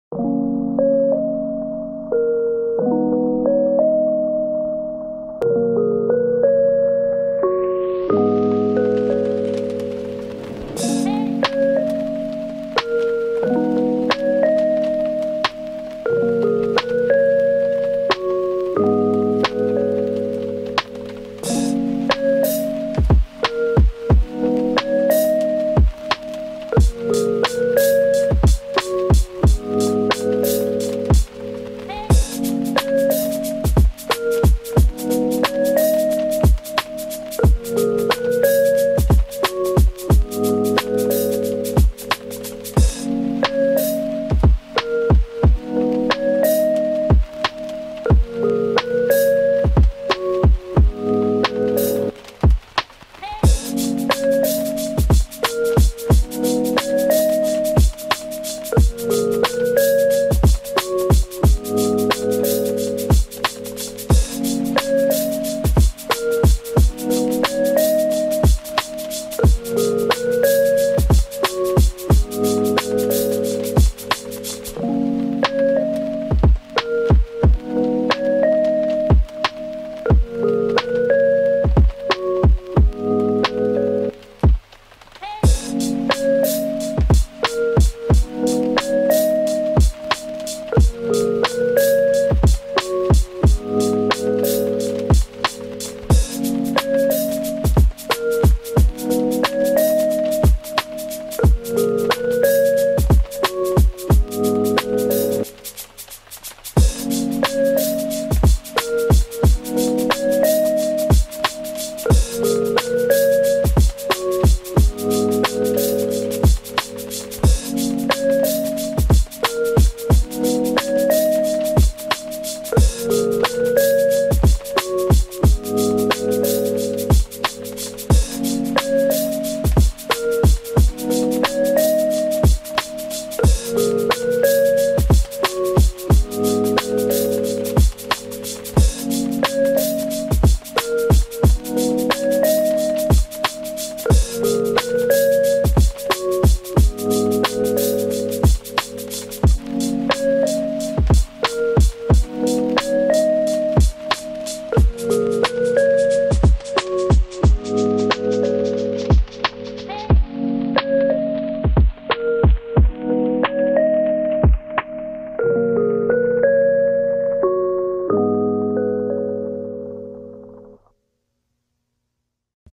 Citizen Fugu Full Lume Marine Promaster Limited Edition - NY0119 19X
Hi

I unbox a new Citizen Fugu Full Lume Marine Promaster Limited Edition - NY0119 19X.
Love the FULL LUME on this watch!